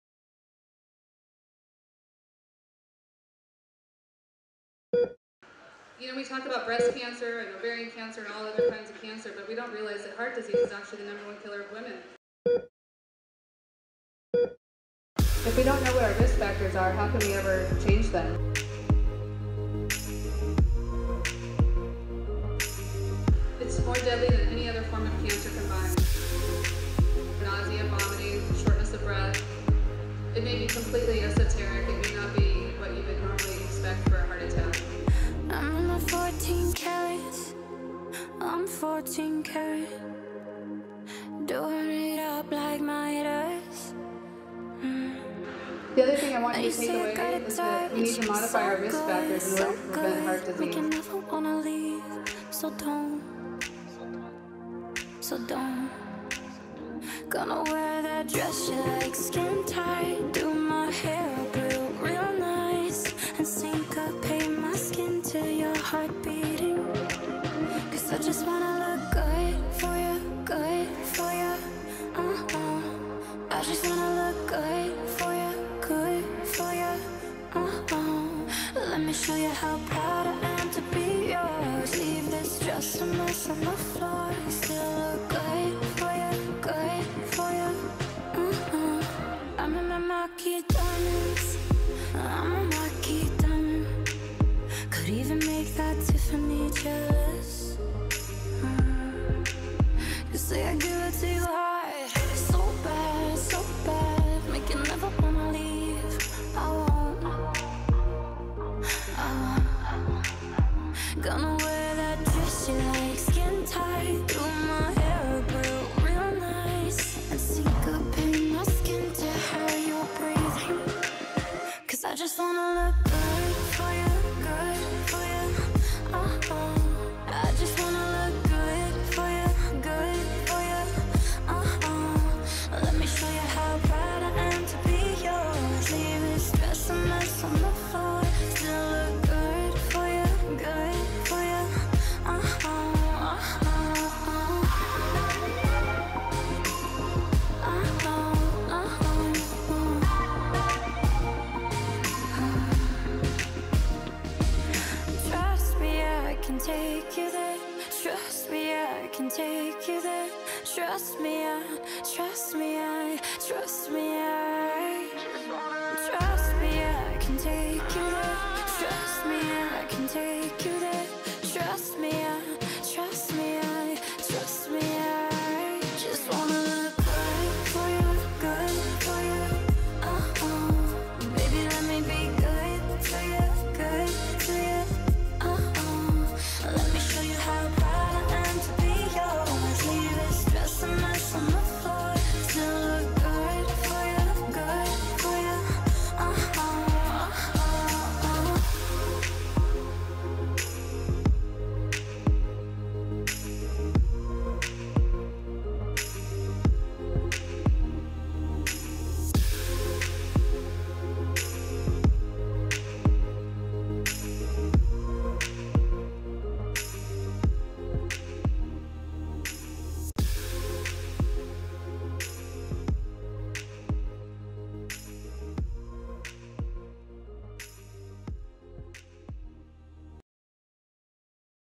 Rock Up In Red Fashion Show
On Feb. 5, service members and personnel assigned to CJTF-HOA and Camp Lemonnier rocked the runway during a fashion show aimed to educate and raise awareness about heart disease in women. If you missed the show, check out this highlight video!